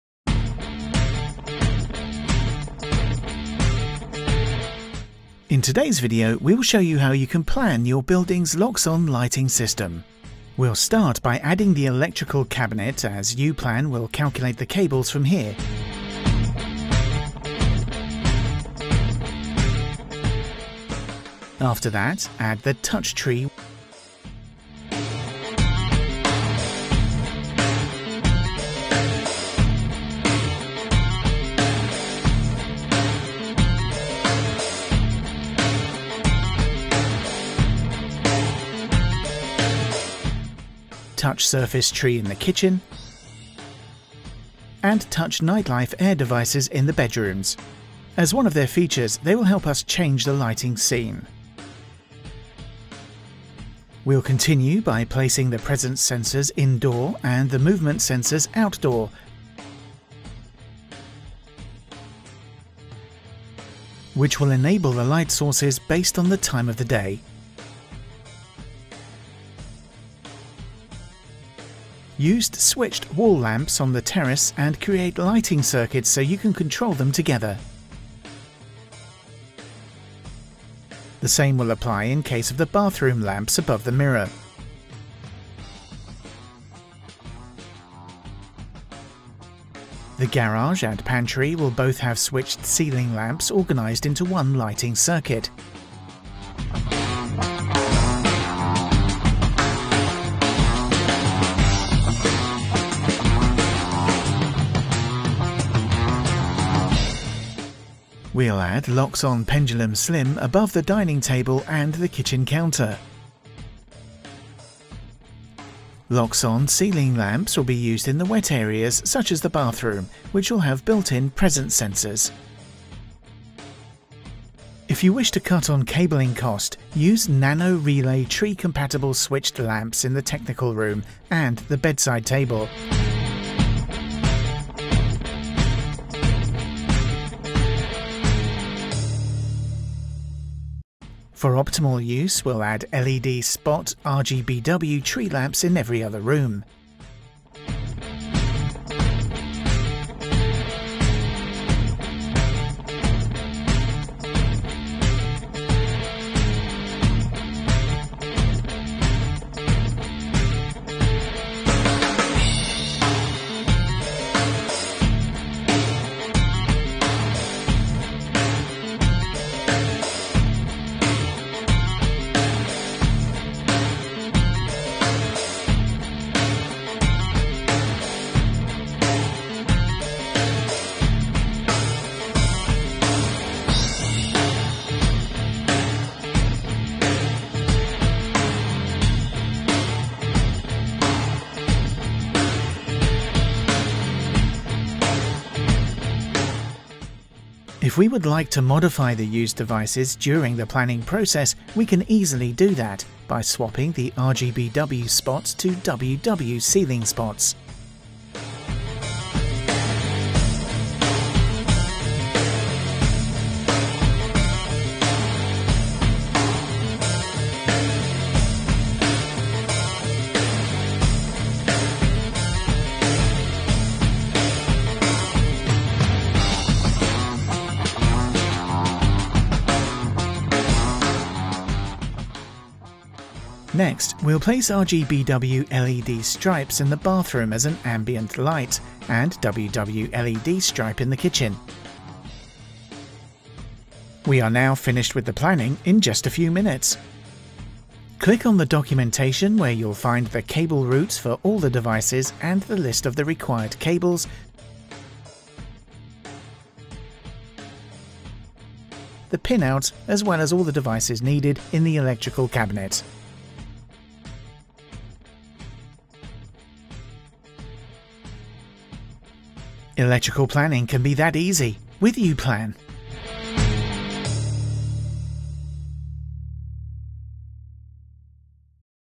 Loxone lighting system planning with uPlan
In this video, we will show you how to plan your Loxone Lighting system.

Learn how to:

- Place the required lights in certain type of rooms

- Where to add switched wall & ceiling lamp, spots, pendulum or LED lights

- Compact solution Loxone has with Presence sensor integrated RGBW lamps

- How you can easily modify any upcoming changes instantly in uPlan

- How you can create a quote & documentation with a single click including all the article numbers, prices, product images etc.